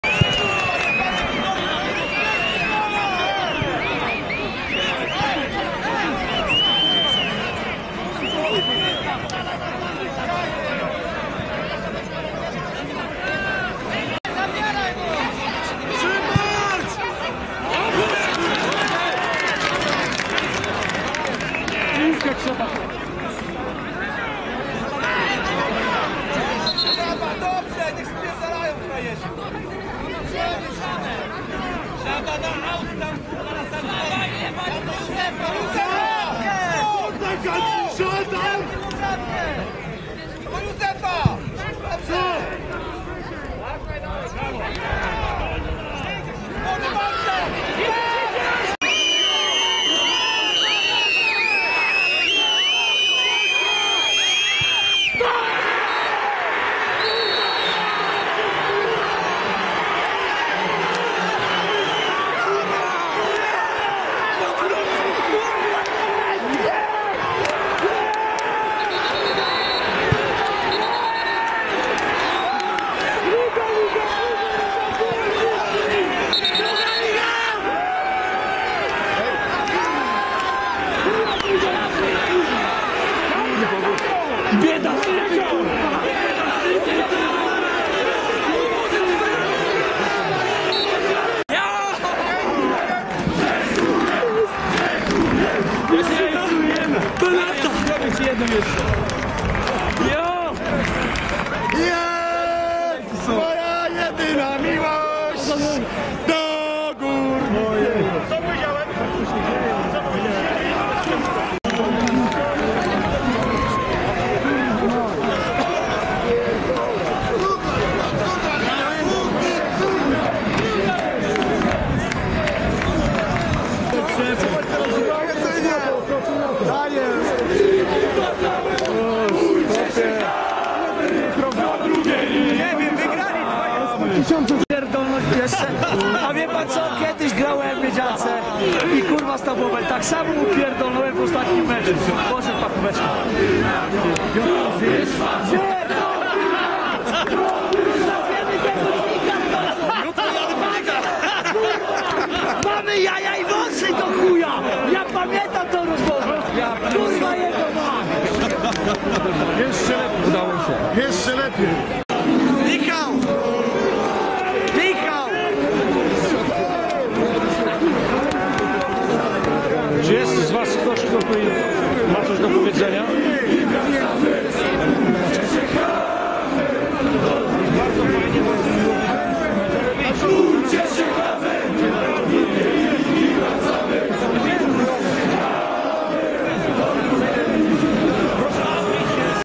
Górnik PWSZ Wałbrzych - awans do drugiej ligi. Więcej: walbrzych.naszemiasto.pl
Górnik PWSZ-Arka Nowa Sól - rzut karny w 94 minucie, który zadecydował o awansie wałbrzyskiej drużyny do drugiej ligi. Gola strzelił Piotr Przerywacz. Autor Dariusz Gdesz. Komentarze zawodników i zdjęcia drużyny we wtorek, 22 czerwca w Panoramie Wałbrzyskiej, która ukazuje się razem z Polską-Gazetą Wrocławską.